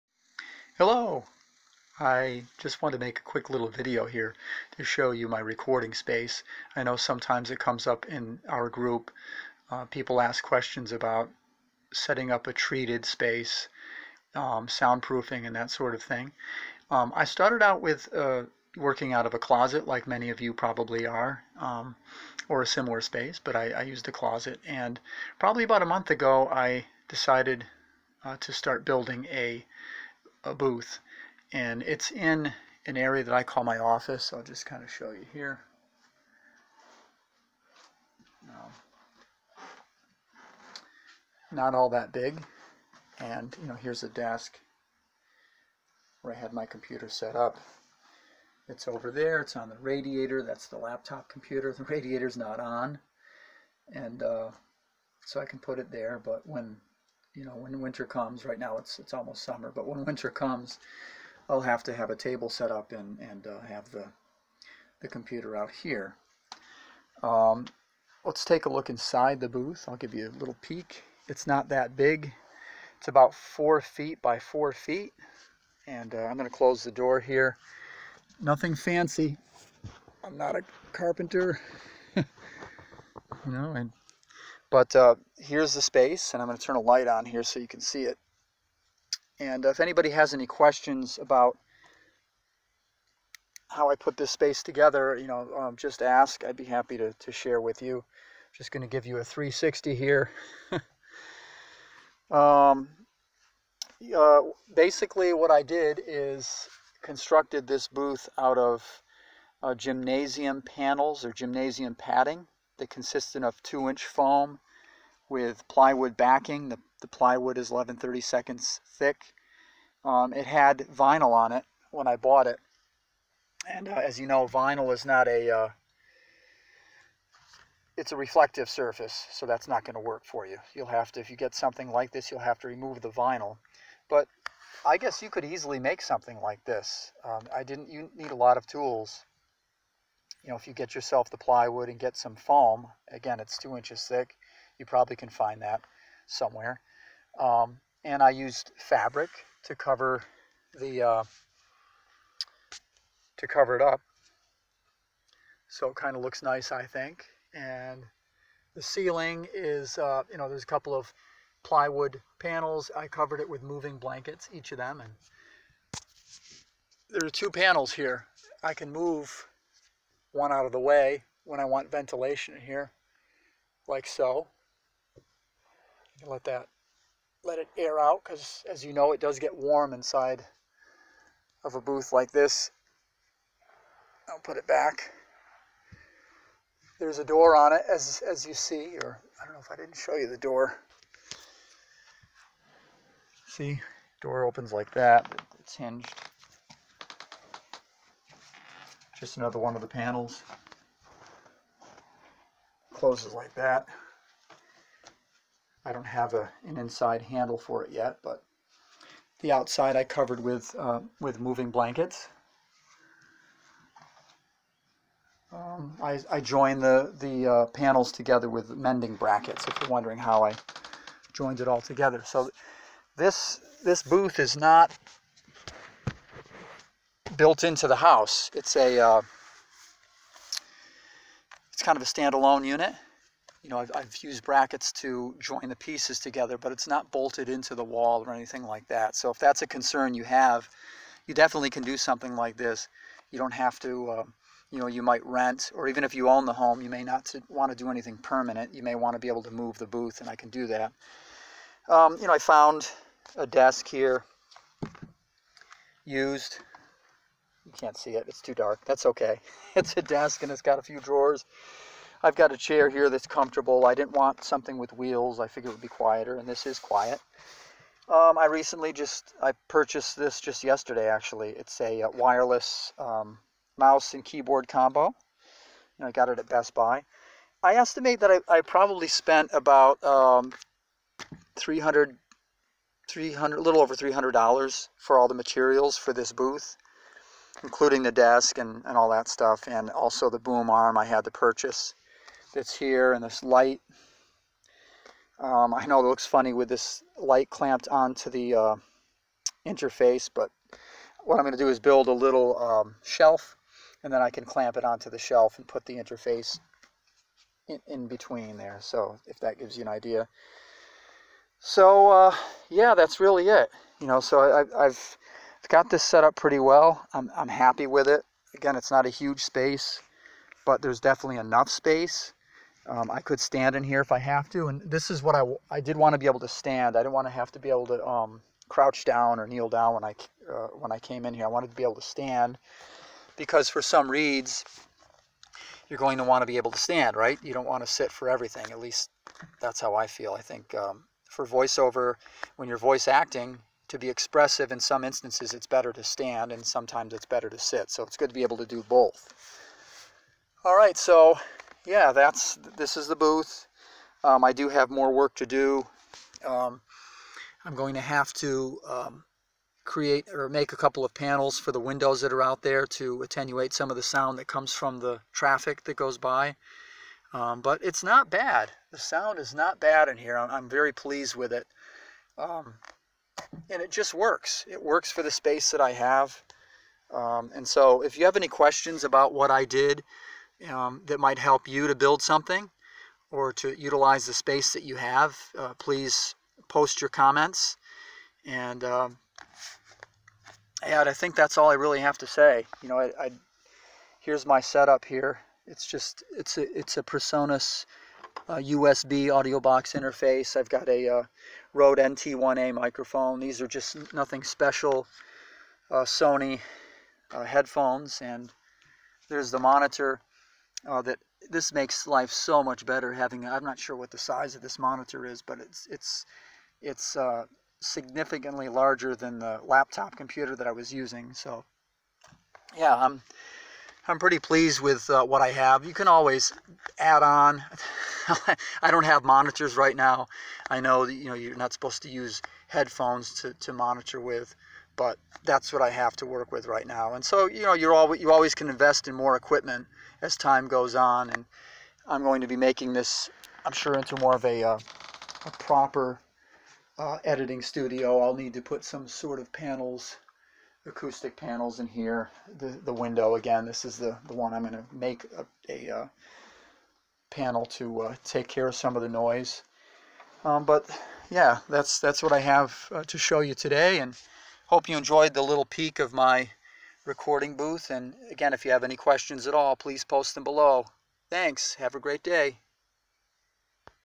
Tour of new recording booth
I recently built my own recording booth which is approximately 4' x 4' with a height of 6'-8".